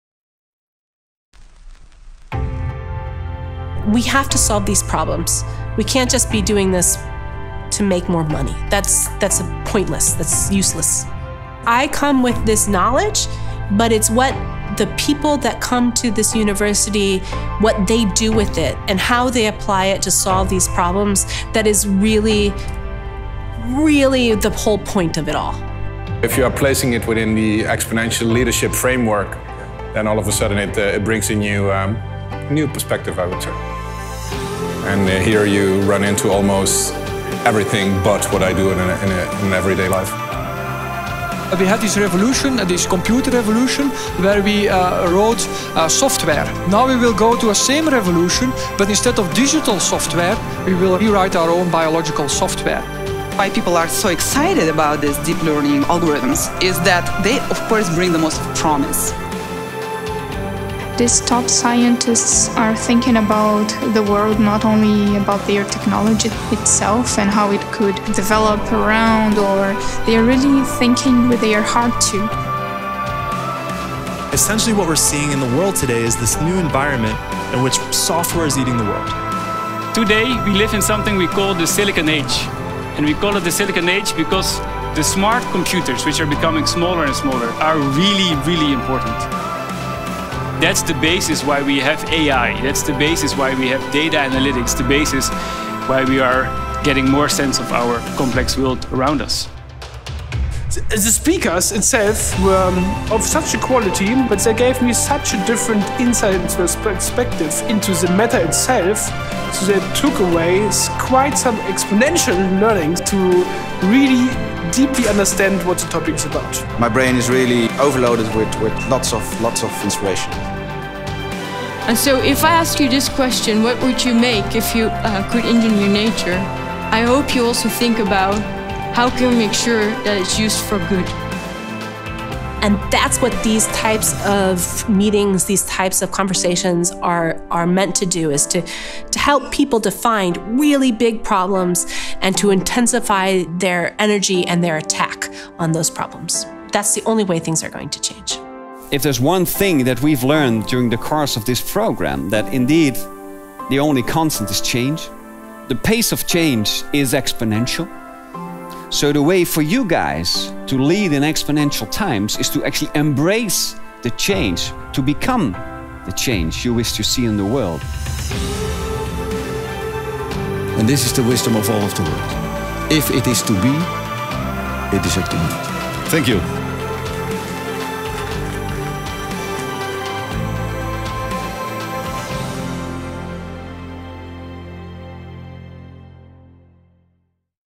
SingularityU NL Executive Program 2018
Guiding leaders and influencers towards creating a lasting impact.

SingularityU The Netherlands Executive Program, an exclusive, 3-day immersive program limited to 50 influential participants. Begin your journey towards Exponential Leadership here.